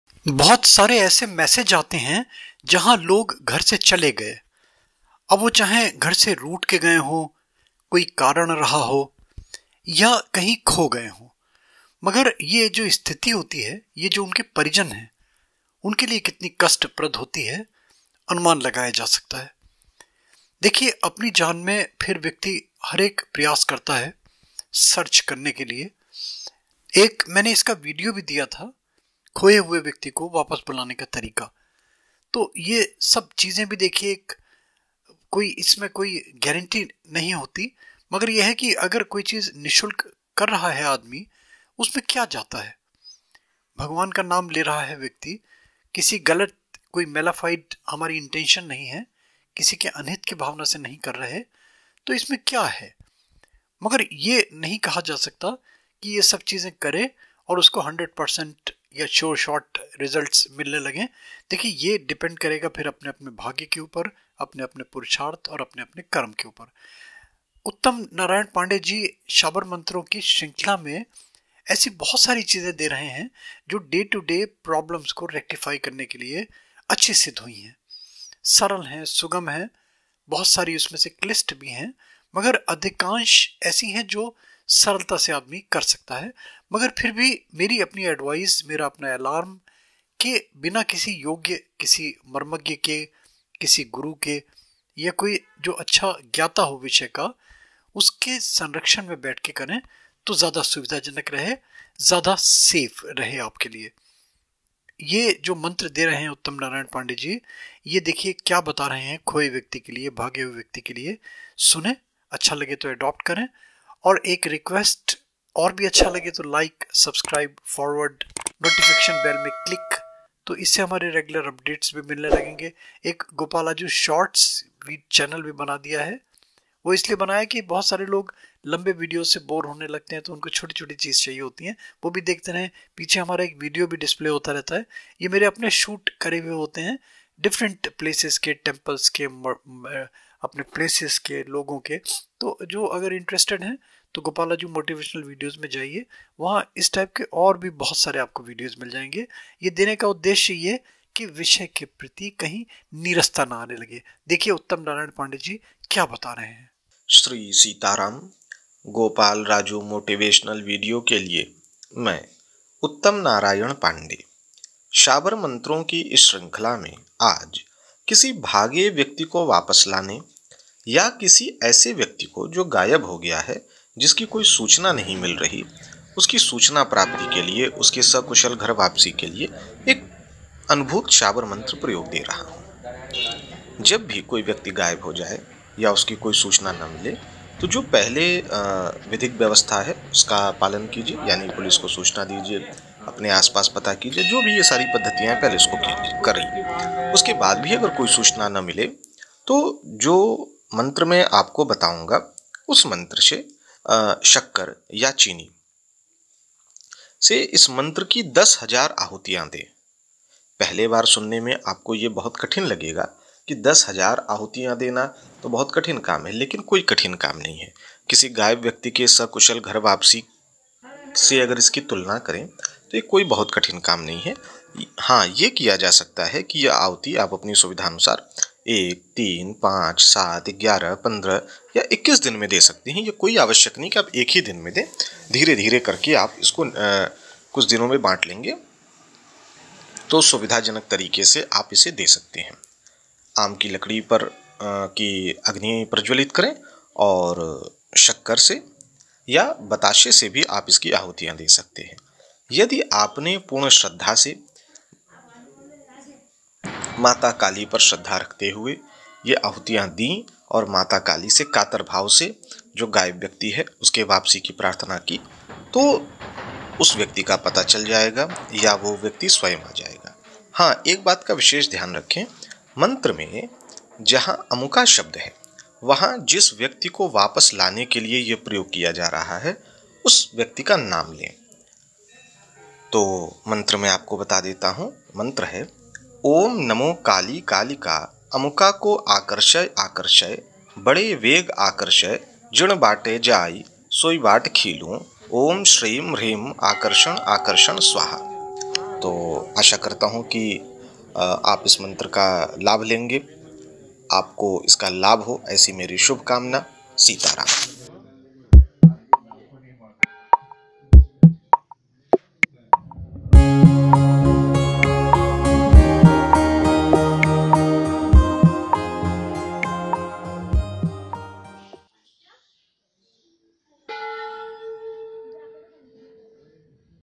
घर से भागा, गुमशुदा, लापता, ग़ायब, अथवा खोये इंसान को बुलाने का उपाय | Remedy for Missing Person
In this video I am telling a powerful remedy for any lost, missing, abscond  or escaped person  from home. May like to see this video also for missing person

👇 घर से भागा,  गुमशुदा, लापता, ग़ायब,  अथवा खोये इंसान को बुलाने का उपाय | Remedy for Missing or abscond Person |

👇 किसी गायब व्यक्ति या भागे हुए व्यक्ति के सकुशल घर वापसी के लिए काली शाबर मन्त्र उपाय विधि -

ऊँ नमो काली कालिका अमुका को आकर्षय आकर्षय बड़े वेग आकर्षय जिण वाटै जाई सोई वाट खीलू ऊँ श्रीं ह्रीं आकर्षण आकर्षण स्वाहा ।।

►► Back Ground Music, Pics, Video Credit & Curtsy  to You Tube & Pixabay  Free  Library